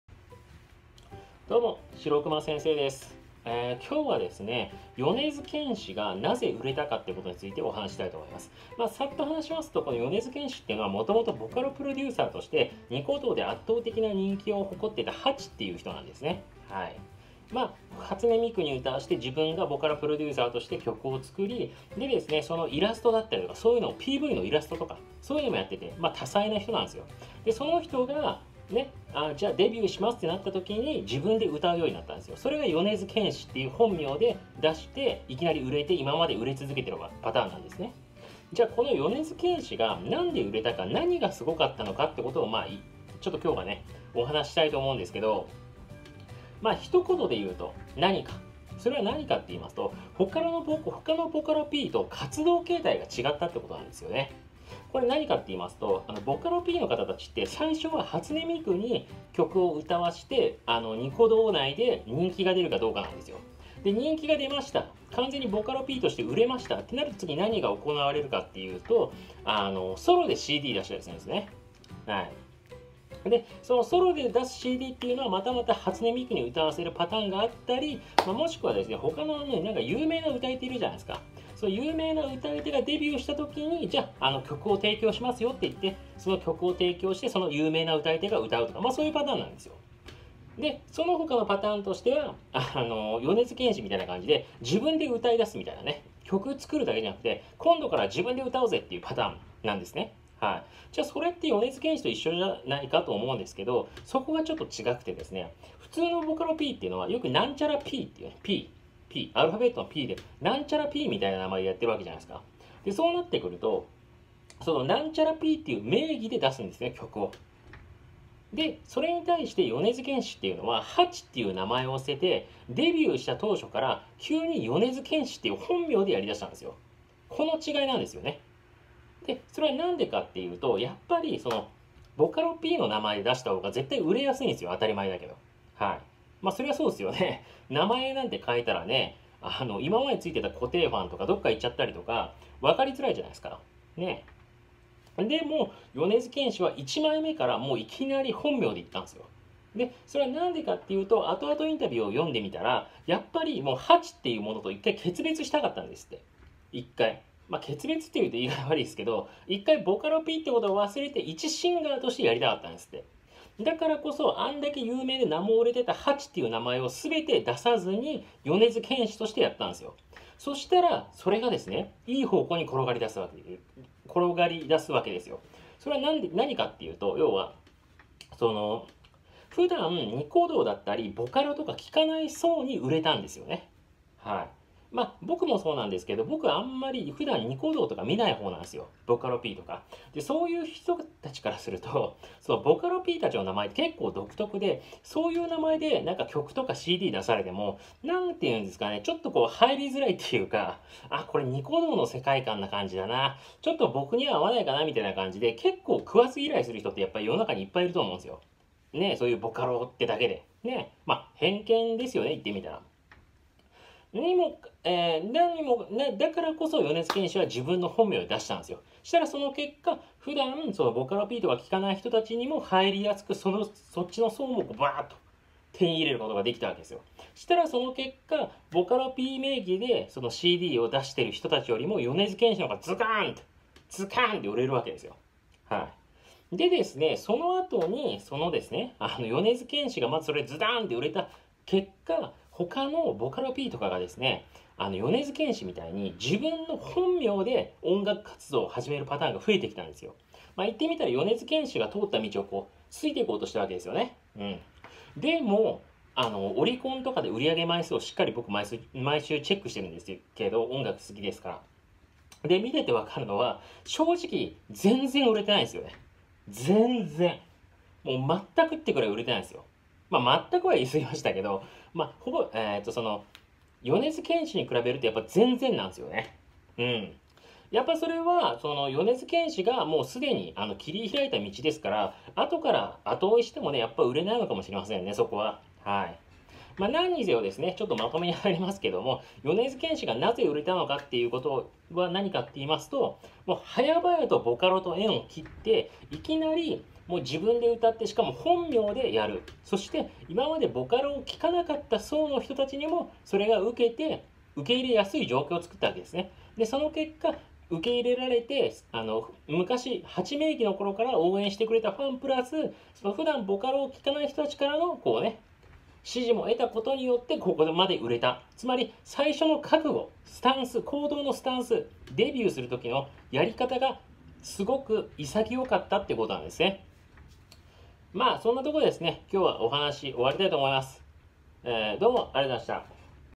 米津玄師がして他のボカロPがしなかったことを解説！
米津玄師がボカロPの中でも広く売れた理由を説明してます！